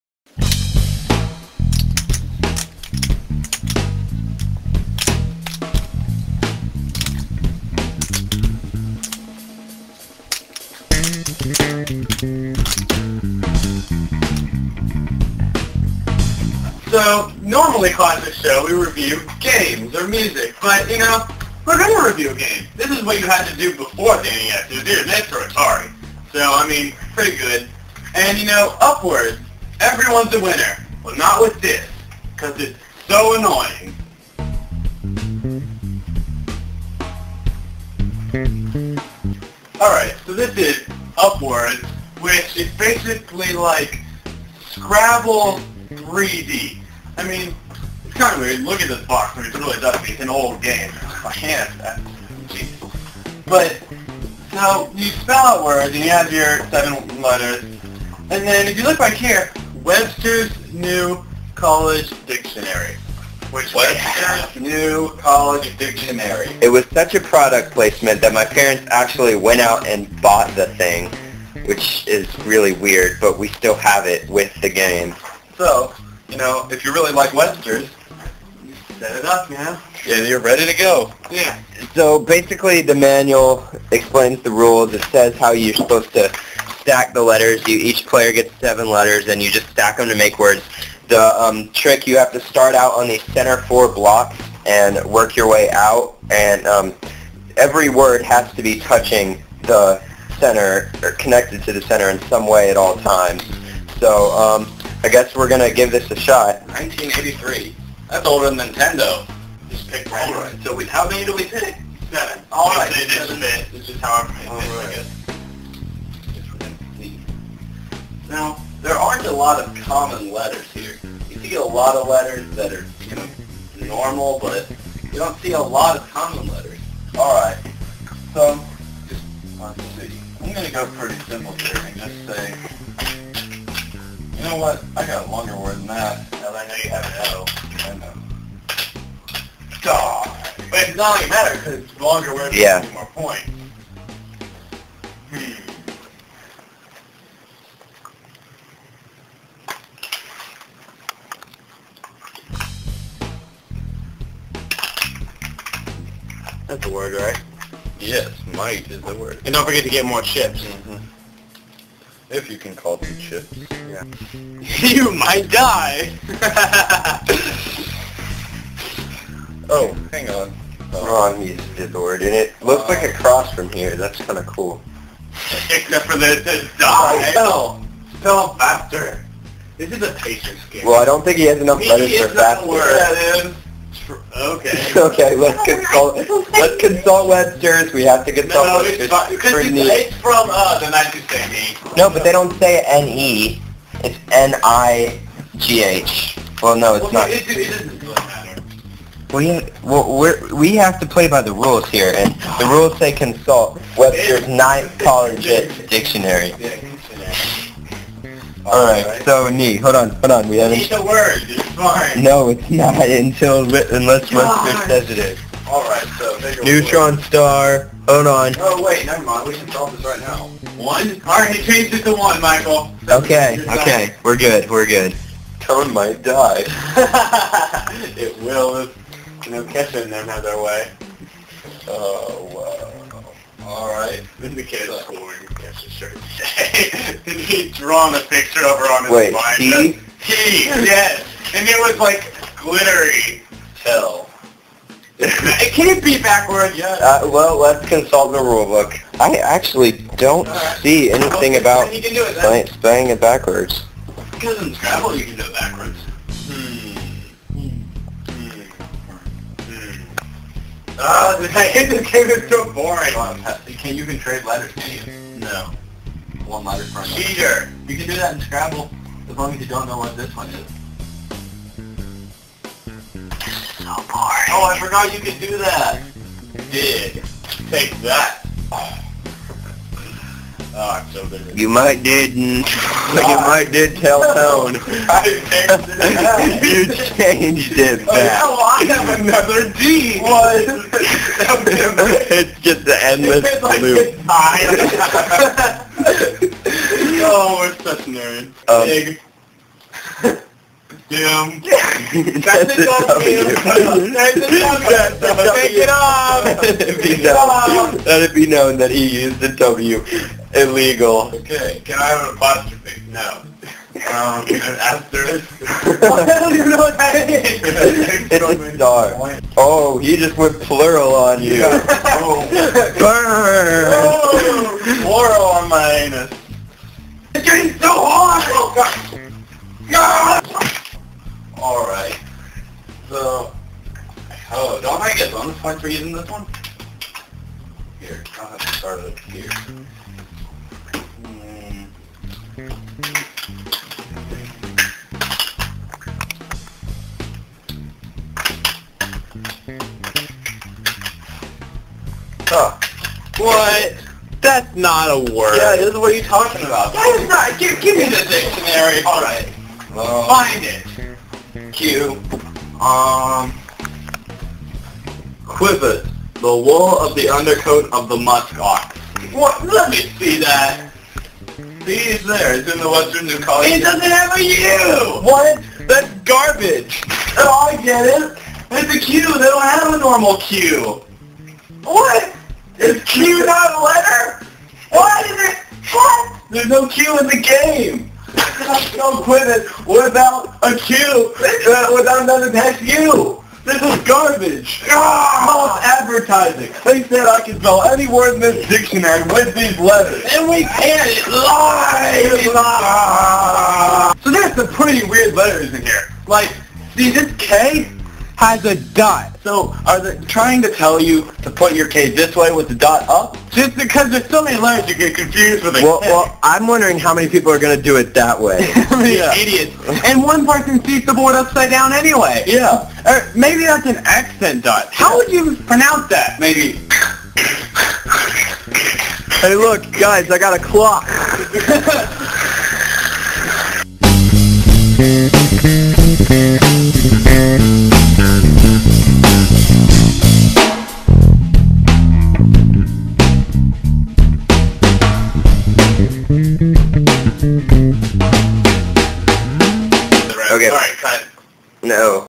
Upwords- Classics Show Board Game Review
Nathan, John and Michael take a swing at reviewing a board game...literally.